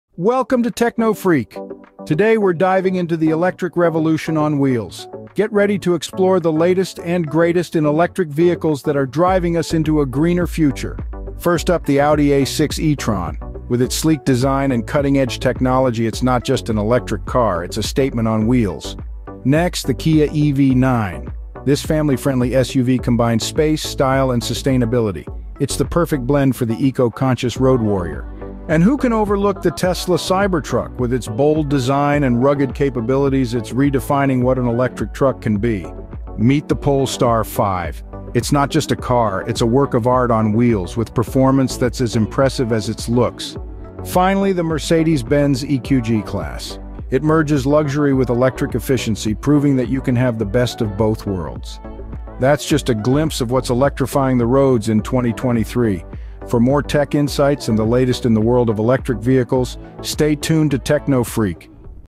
Exploring 2023's Electrifying Innovations: The Future of Electric Vehicles
🚗 Welcome to TecnoFreak! In today's episode, we're exploring the electrifying world of the latest electric vehicles 🌍. Join us as we journey through 2023's most innovative and eco-friendly rides 🚀. From the sleek Audi A6 e-Tron to the rugged Tesla Cybertruck, we're covering it all. Discover the perfect blend of luxury, sustainability, and high performance with models like the Kia EV9, Polestar 5, and Mercedes-Benz EQG-Class 🌟.

In this video, you'll discover:

The Audi A6 e-Tron: More than just an electric car, a statement on wheels 🏎️.
The Kia EV9: The ideal eco-friendly family SUV 🌿.
The Tesla Cybertruck: Redefining the electric truck 🛻.
The Polestar 5: A work of art meeting high performance 🎨.
The Mercedes-Benz EQG-Class: The epitome of luxury and electric efficiency 💎.
Why Watch?
Whether you're a car enthusiast, an eco-conscious consumer, or just love keeping up with the latest tech trends, this video is for you 🌐. Get ready to be amazed by the advancements in electric vehicle technology that are setting new standards for the automotive industry 🌟.

Share Your Thoughts:
Which electric vehicle caught your eye? Any specific model you want us to review next? 💬

SPANISH:
🚗 ¡Bienvenidos a TecnoFreak! En el episodio de hoy, exploramos el emocionante mundo de los vehículos eléctricos más recientes 🌍. Acompáñanos en un viaje por los modelos más innovadores y ecológicos de 2023 🚀. Desde el elegante Audi A6 e-Tron hasta el robusto Tesla Cybertruck, lo cubrimos todo. Descubre la combinación perfecta de lujo, sostenibilidad y alto rendimiento con modelos como el Kia EV9, el Polestar 5 y el Mercedes-Benz EQG-Class 🌟.

En este video, descubrirás:

El Audi A6 e-Tron: Más que un coche eléctrico, una declaración sobre ruedas 🏎️.
El Kia EV9: El SUV familiar ideal y ecológico 🌿.
El Tesla Cybertruck: Redefiniendo el camión eléctrico 🛻.
El Polestar 5: Una obra de arte con alto rendimiento 🎨.
El Mercedes-Benz EQG-Class: El epítome del lujo y la eficiencia eléctrica 💎.
¿Por qué verlo?
Tanto si eres un entusiasta de los coches, un consumidor consciente del medio ambiente o simplemente te encanta estar al día con las últimas tendencias tecnológicas, este video es para ti 🌐. Prepárate para sorprenderte con los avances en tecnología de vehículos eléctricos que están estableciendo nuevos estándares en la industria automotriz 🌟.

Comparte Tus Opiniones:
¿Qué vehículo eléctrico captó tu atención? ¿Hay algún modelo específico que quieras que revisemos a continuación? ¡Deja tus comentarios abajo! 💬